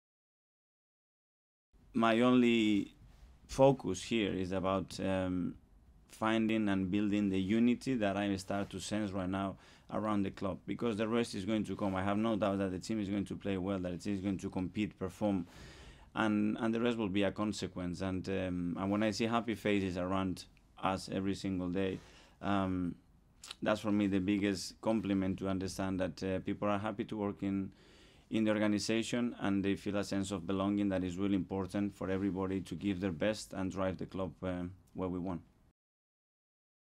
EPL | Arteta focus on 'building unity' as he reaches 100 games as Arsenal boss.
Arsenal manager Mikel Arteta said his only focus as he passes 100 games in charge at the Emirates Stadium is "finding and buidling the unity" at the club.

"I have no doubt that the team is going to play well, that is going to compete, perform, and the rest will be a consequence, and I want to see happy faces around us every single day," said Arteta.